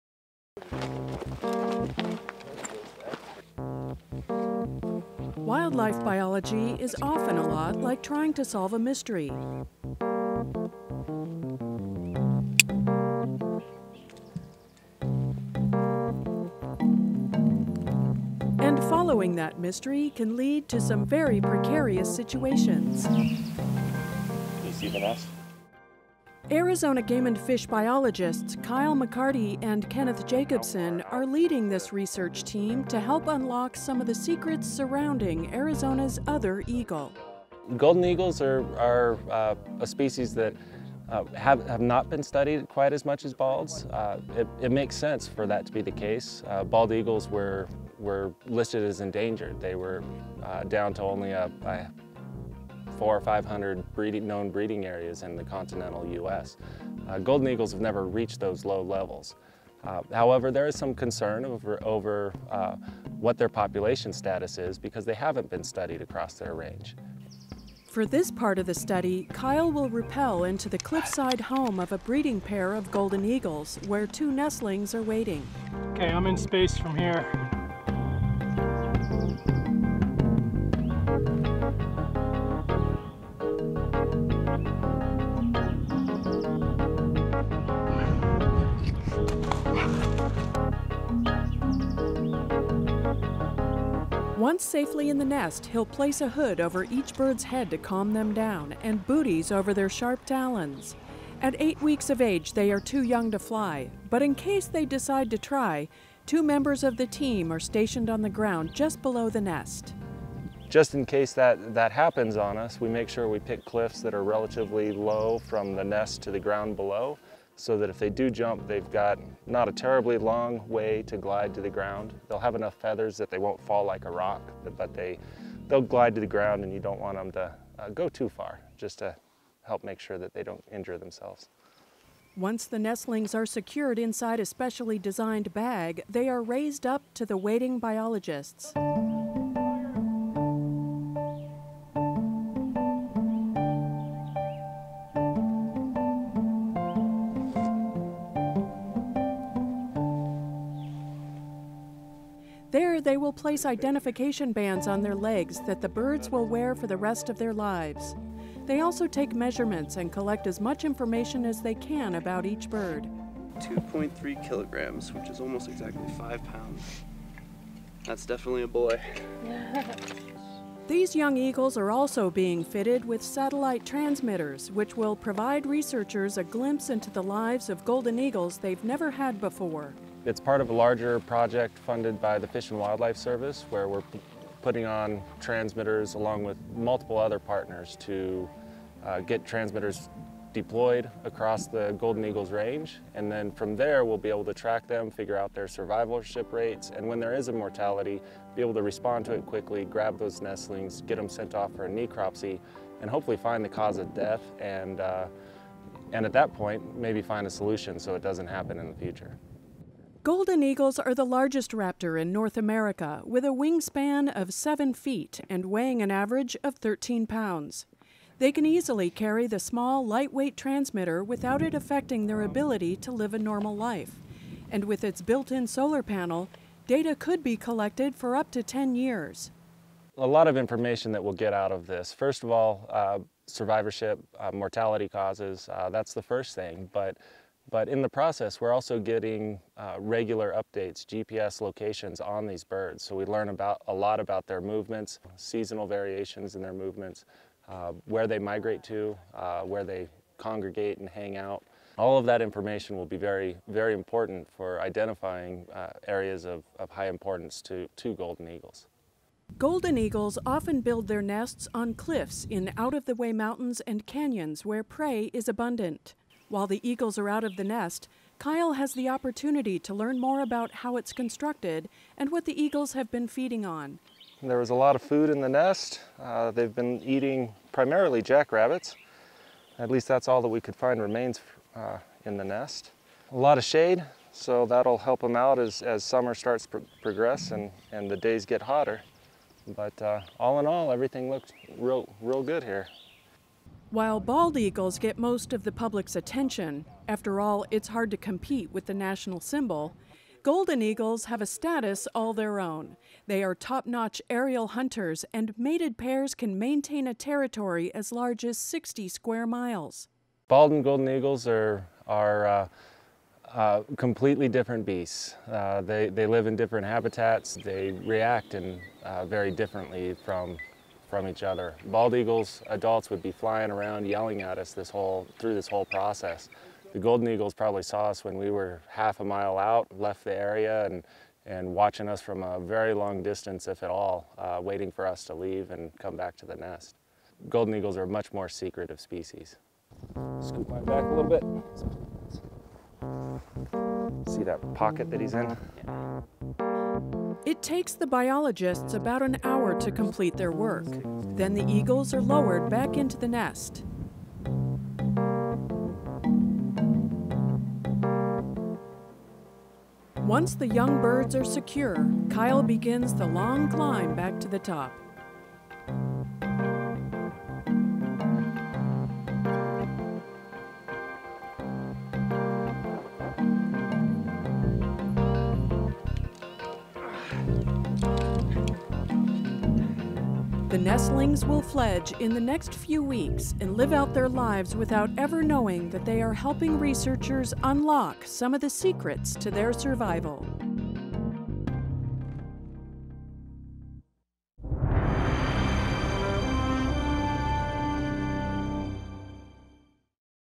Golden Eagle Study
Arizona Game and Fish biologists are studying Golden Eagles to learn more about this secretive raptor. This video was created by the Information Branch of the Arizona Game and Fish Dept. Producers: Carol Lynde and David Majure.